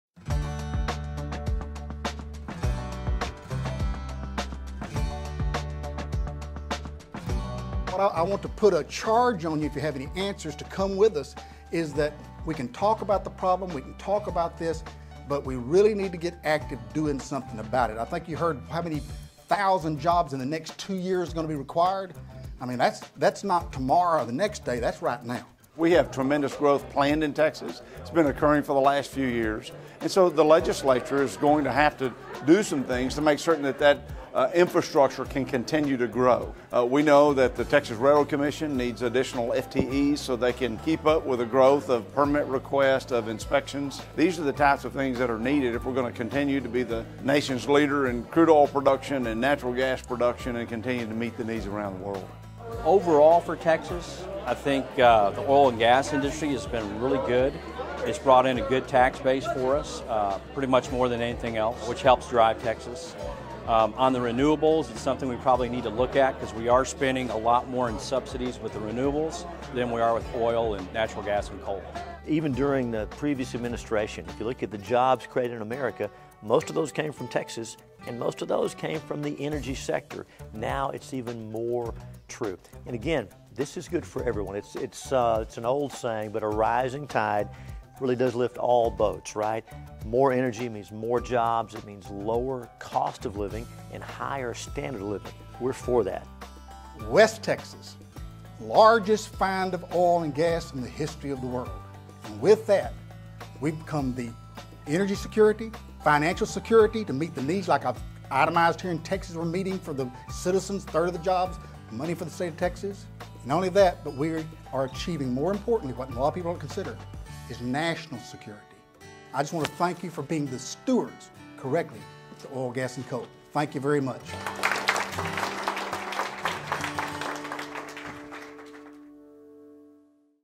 Commissioner Christian Leads 86th Legislature Energy Summit
See what three important factors @TxRRC Commissioner Wayne Christian says we should all keep in mind at this year's 86th Legislative Summit.